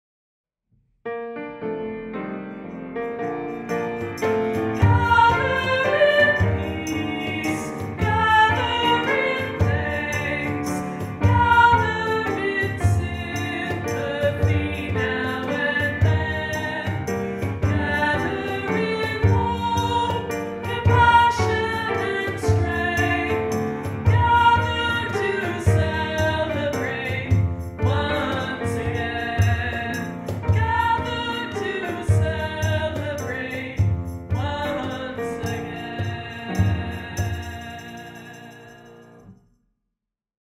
Neighbourhood Welcome Video
A short video used to begin services at Neighbourhood Unitarian Universalist Congregation. It features minister Rev Wayne Walder, music director Susanne Maziarz and child and caregiver director Zee Getchell.